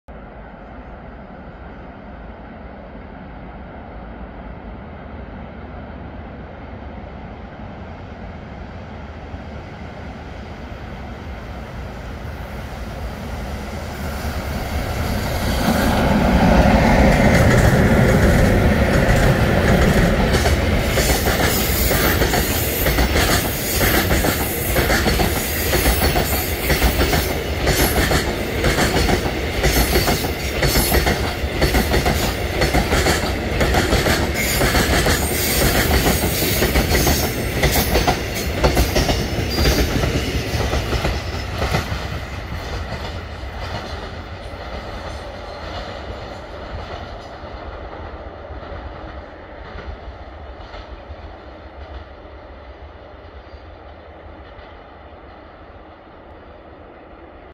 태백선 태백 삼각선을 통과하는 중련 시멘트 화물열차
촬영일자 및 시간
2022년 2월 14일 월요일 14:21:56

촬영 장소
강원도 태백시 백산동

운행 정보
동해》수색

시간이 조금 지났지만
2월 눈 날리는 날 촬영했던 벌크시멘트 화물열차입니다.

이곳의 선로는 태백선과 영동선으로 갈라지는 곳인 삼각선이기도 하죠.
솔안터널이 없던 시절엔 제 뒤편으로 선로가 하나 더 있는데 그쪽으로도 운행을 했었습니다.
추후에 영상 올릴때 삼각선의 구조와 열차도 올리겠습니다.

좋은 하루 좋은 시간 되세요~~☕️🍀🌹